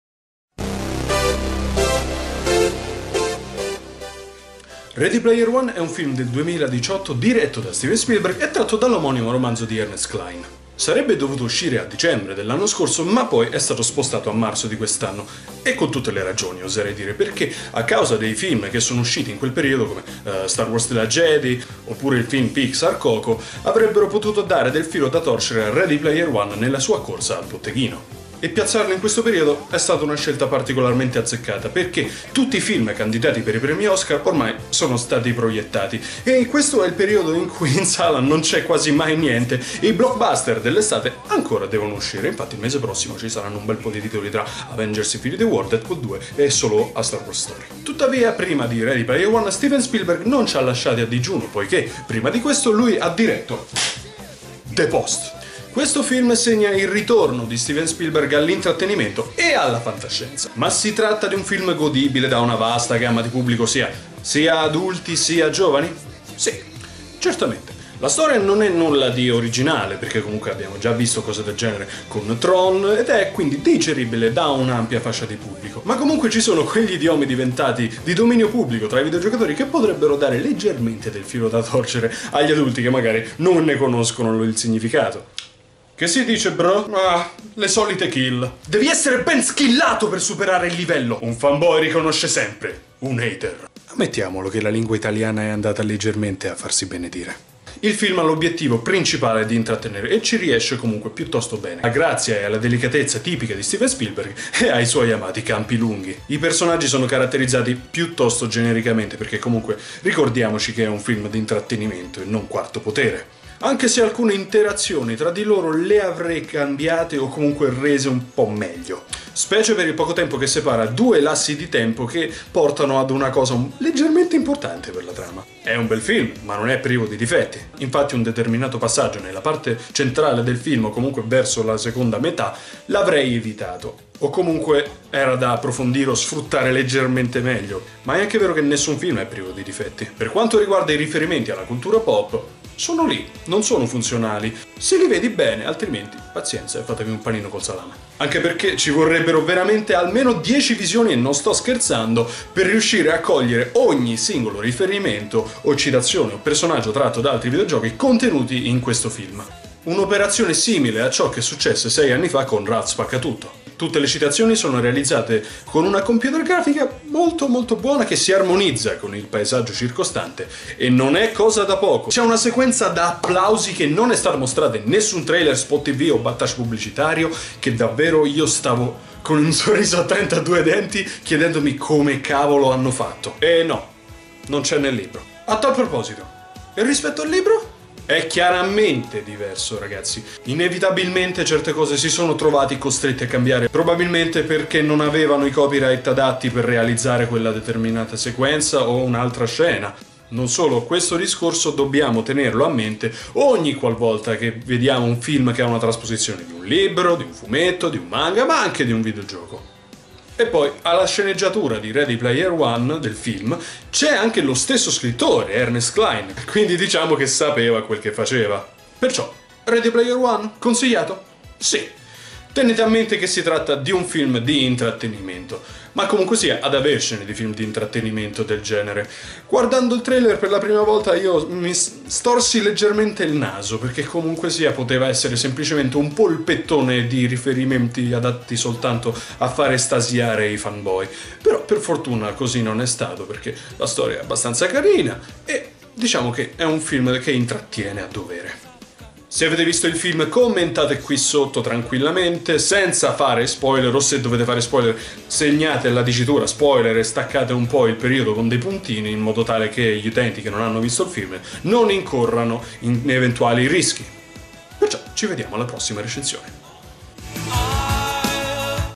Ready Player One: video-recensione | 17KGroup.it
Un ritorno all'intrattenimento da parte dellp zio Spielberg"
Videorecensione di Ready Player One (2017), basato sull'omonimo romanzo di Ernest Cline.
A cura di:
Giulio Bruschini
Pierfrancesco Lucchetti
Francesco Barone

Seguici nei nostri canali social!